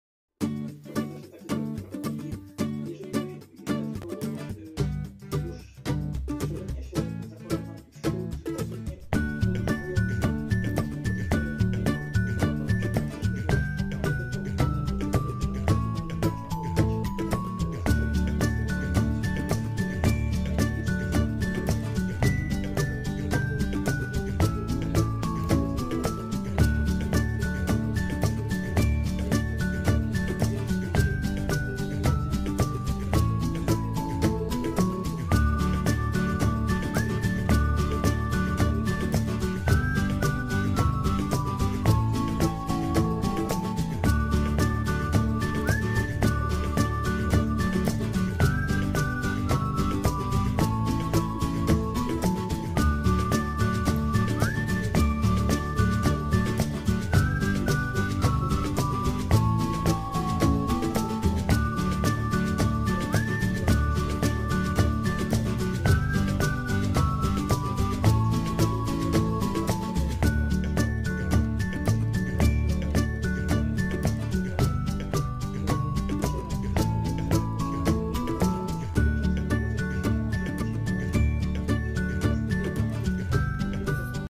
Silk painting: how the outlines are painted in Flying Dragon scarf
The painting process of Flying Dragons silk scarf, done by MinkuLUL Luiza Malinowska.
Using silk paints and thin brush I am able to mask white gutta lines with colors.